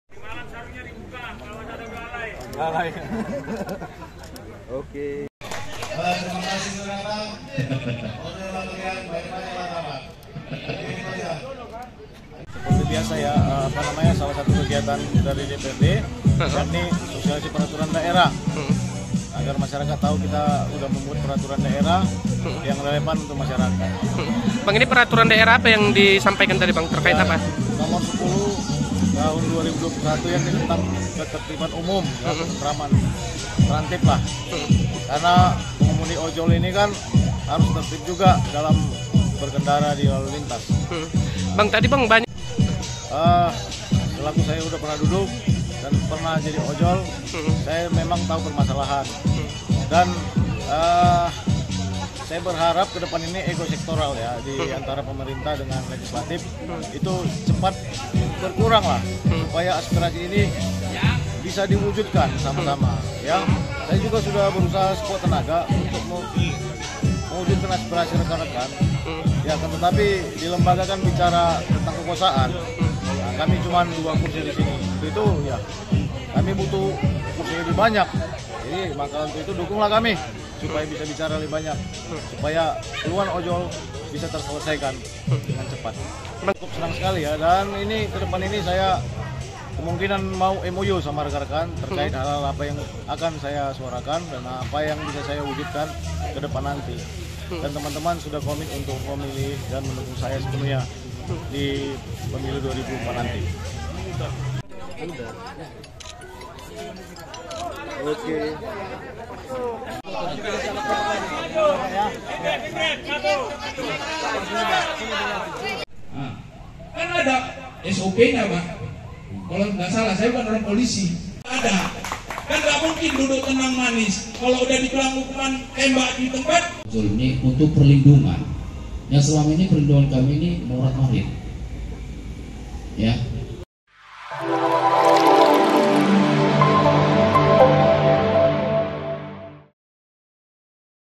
Gelar Sosialisasi Perda Kenakan Jaket Ojol, Anggota DPRD Medan Erwin Siahaan Didukung Kembali Maju
TRIBUN-MEDAN.com, MEDAN - Mengenakan jaket driver ojek online, Anggota DPRD Medan dari Partai Solidaritas Indonesia (PSI), Erwin Siahaan menggelar Sosialisasi Peraturan Daerah Kota Medan Nomor 10 tentang Ketentraman Umum, Sabtu (9/9/2023).

Dihadapan ratusan masyarakat dan driver ojek online, Erwin Siahaan mengajak agar masyarakat dan pengemudi ojek online menjadi garda terdepan dalam menjaga ketertiban dan keselamatan saat berkendara.

"Ini adalah sosialisasi soal peraturan daerah tentang ketertiban umum. Aturan yang sudah kami DPRD rancang untuk diterapkan di masyarakat. Khususnya bagi pengemudi ojek online yang setiap hari di jalan agar mematuhi aturan untuk menjaga keselamatan," kata Erwin.